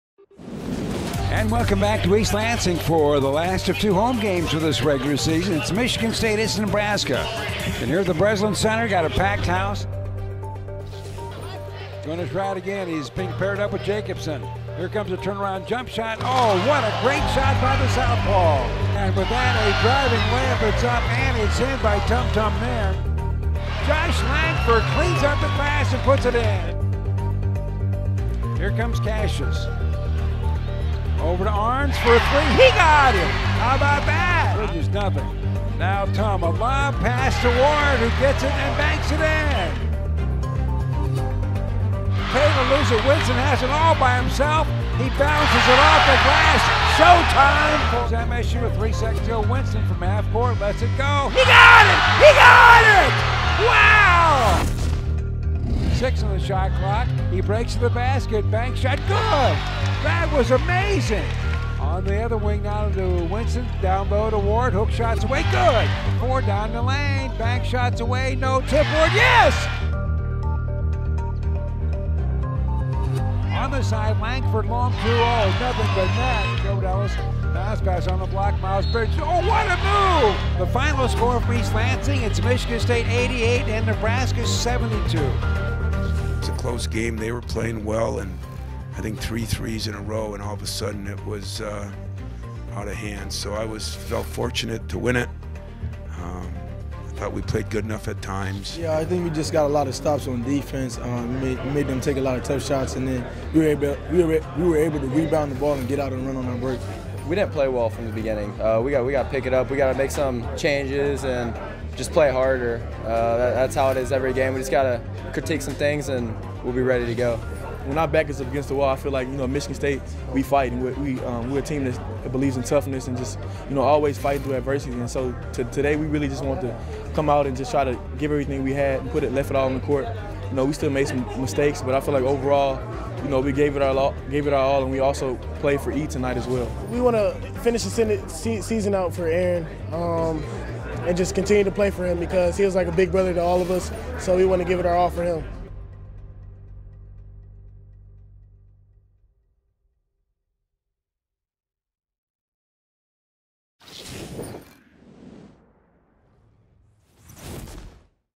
Michigan State beats Nebraska 88-72
Nick Ward had 20 points, giving Michigan State two freshmen with 20-plus points in a game for the first time since the 1977-78 season when Magic Johnson scored 31 and Jay Vincent had 22 points.

Joshua Langford scored a career-high 17 points as the Spartans (17-11, 9-6 Big Ten ) bounced back from a 17-point setback at No. 14 Purdue and the loss of senior Eron Harris with a dominant performance in a game they needed to win.